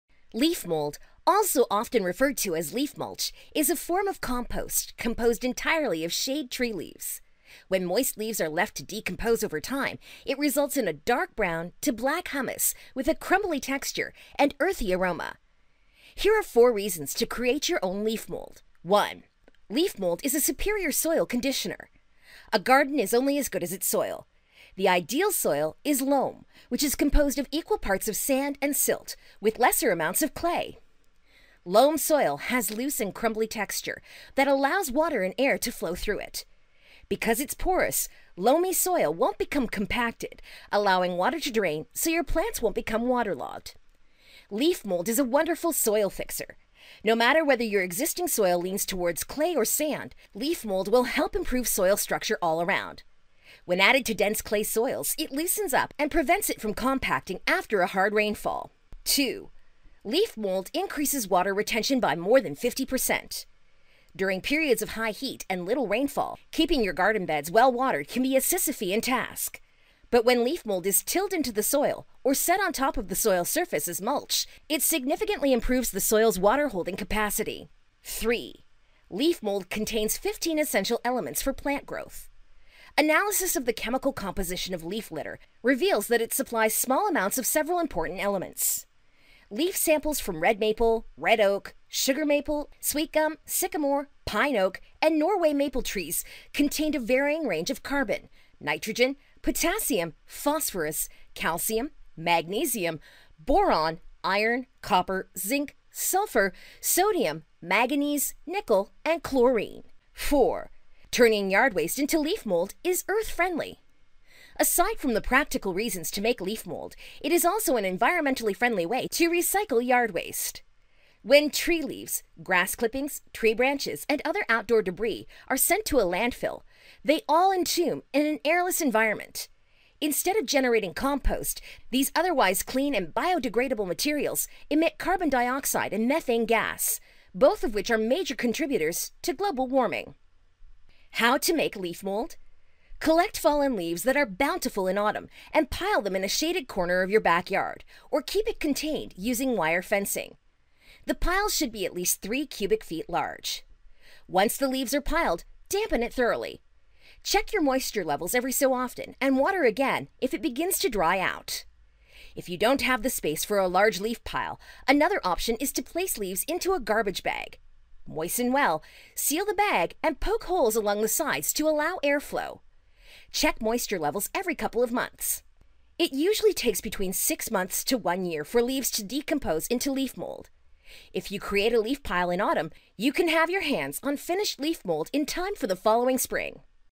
Amazing Reasons To Use Leaf Mould And How To Make It At Home.
Leaf-mold, also often referred to as leaf mulch, is a form of compost composed entirely of shade tree leaves. When moist leaves are left to decompose over time, it results in a dark brown to black humus with a crumbly texture and earthy aroma.
Here are 4 REASONS TO CREATE YOUR OWN LEAF MOLD

1.LEAF MOLD IS A SUPERIOR SOIL CONDITIONER
A garden is only as good as its soil. The ideal soil type is loam, which is composed of equal parts sand and silt with lesser amounts of clay.
Loam soil has a loose and crumbly texture that allows water and air to flow through it. Because it’s porous, loamy-soil won’t become compacted, allowing water to drain so your plants won’t become waterlogged.
Leaf-mold is a wonderful soil-fixer. No matter whether your existing soil leans toward clay or sand, leaf mold will help improve soil structure all around. When added to dense clay soils, it loosens it up and prevents it from compacting after a hard rainfall.

2.LEAF MOLD INCREASES WATER RETENTION BY MORE THAN 50%.
During periods of high heat and little rainfall, keeping your garden beds well watered can be a Sisyphean task. But when leaf-mold is tilled into the soil or set on top of the soil surface as a mulch, it significantly improves the soil’s water holding capacity.

3.LEAF MOLD CONTAINS 15 ESSENTIAL ELEMENTS FOR PLANT GROWTH
Analysis of the chemical composition of leaf litter reveals that it supplies small amounts of several important elements.
Leaf samples from red maple, red-oak, sugar maple, sweet-gum, sycamore, pin oak, and Norway maple-trees contained a varying range of carbon, nitrogen, potassium, phosphorous, calcium, magnesium, boron, iron, copper, zinc, sulfur, sodium, manganese, nickel, and chlorine.

4.TURNING YARD WASTE INTO LEAF MOLD IS EARTH FRIENDLY
Aside from the practical reasons to make leaf mold, it is also an environmentally friendly way to recycle yard-waste.
When tree leaves, grass clippings, tree branches, and other outdoor debris are sent to a landfill, they are entombed in an airless environment. Instead of generating compost, these otherwise clean and biodegradable materials emit carbon-dioxide and methane gas – both of which are major contributors to global-warming.

HOW TO MAKE LEAF MOLD?
Collect fallen leaves that are bountiful in autumn and pile them in a shaded corner of your backyard or keep it contained using wire fencing. The pile should be at least three cubic feet large. Once the leaves are piled, dampen it thoroughly. Check moisture levels every so often and water again if it begins to dry-out.
If you don’t have the space for a large leaf-pile, another option is to place leaves into a garbage bag. Moisten well, seal the bag, and poke holes along the sides to allow airflow. Check moisture-levels every couple months.
It usually takes between six months to one year for leaves to decompose into leaf-mold. If you create a leaf-pile in autumn, you can have your hands on finished leaf mold in time for the following spring.